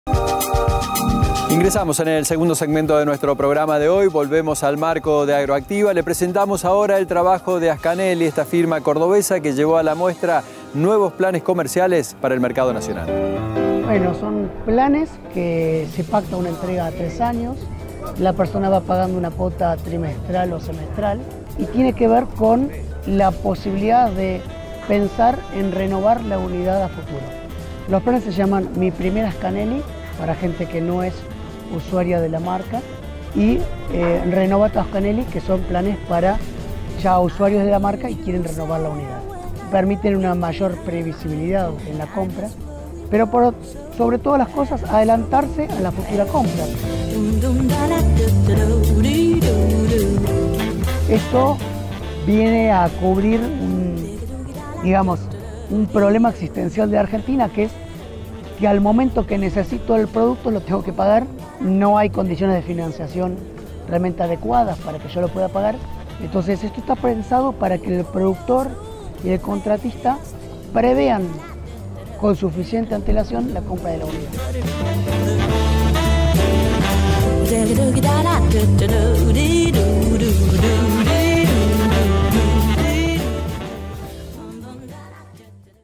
Agroactiva 2019 - Ascanelli en la Muestra
Se presentó en  Agroactiva el nuevo plan de financiación: Mi Primera Ascanelli